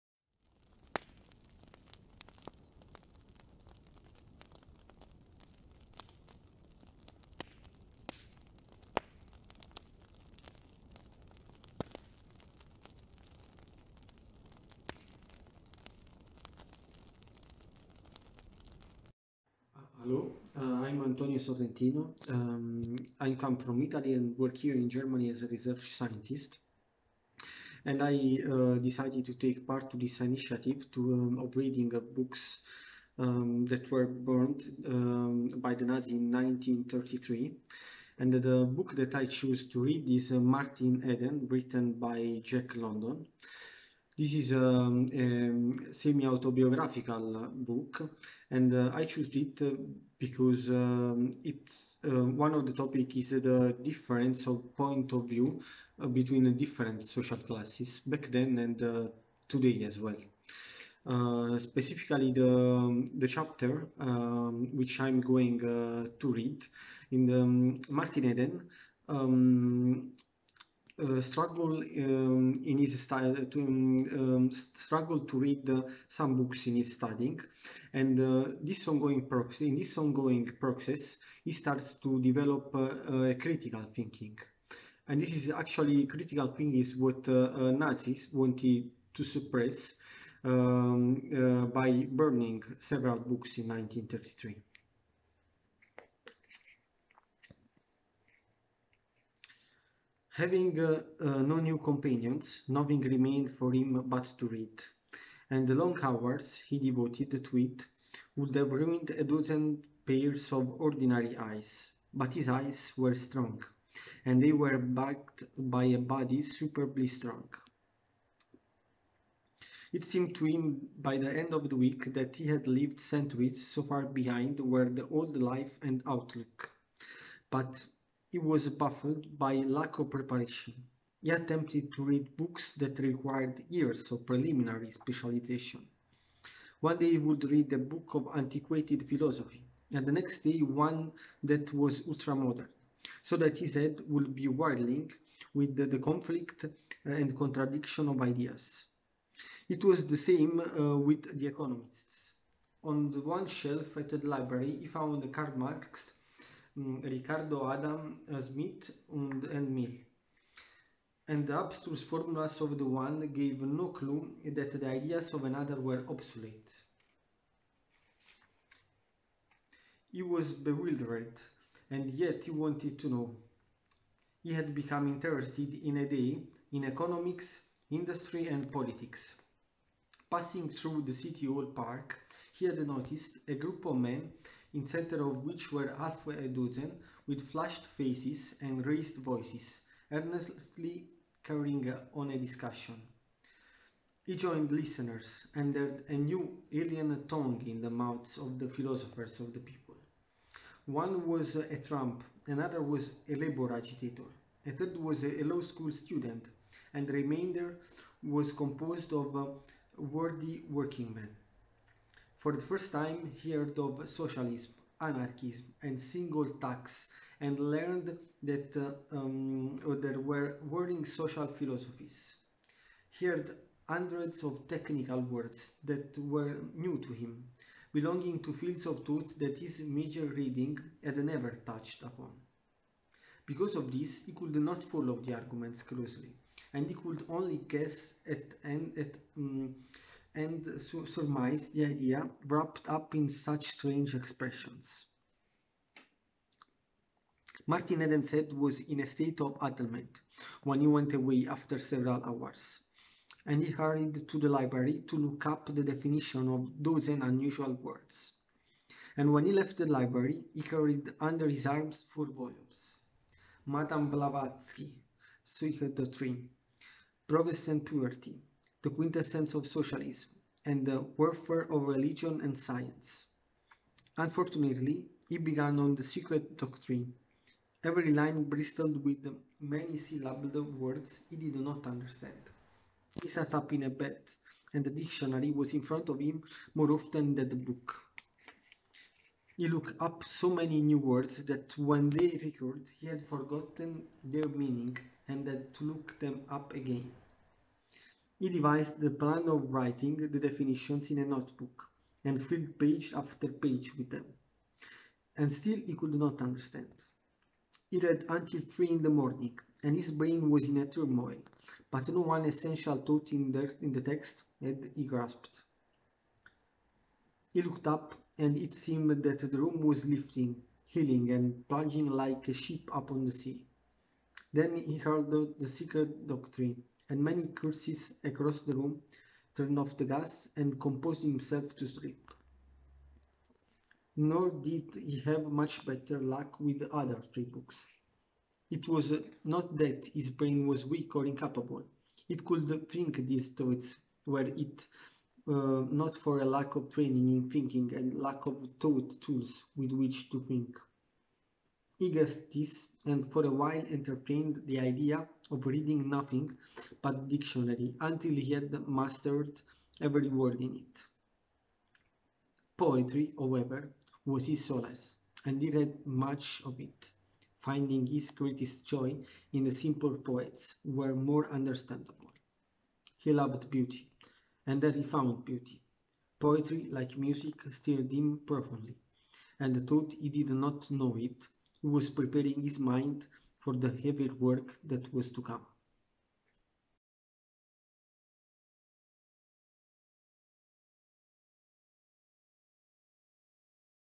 Lesung aus verbrannten Büchern, 22.5.2020 - Antonio Sorrentino liest aus Jack Londons "Martin Eden"
Verbrannte Bücher, verbotene Autoren – Lesung zur Erinnerung an die Bücherverbrennung in Potsdam am 22. Mai 1933

BürgerInnen Potsdams, Menschen aus Politik und Gesellschaft lesen aus Werken von AutorInnen, die von den Nationalsozialisten verboten wurden und deren Bücher ein Raub der Flammen wurde.

Antonio Sorrentino, Mitarbeiter des Max Planck Instituts Magedeburg liest aus Jack Londons "Martin Eden" (englisch mit engl. UT).

Auch nicht-deutsche Autoren wie der Amerikaner Jack London waren unter denen, deren Bücher verbrannt wurden. Dabei war London (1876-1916) bereits lange vor Erstarken des Nationalsozialismus gestorben. Vermutlich brachte seine sozialistische Einstellung die Nazis gegen seine Bücher auf, die schon zu seinen Lebzeiten Bestsellerstatus erreichten und bis heute bekannt sind.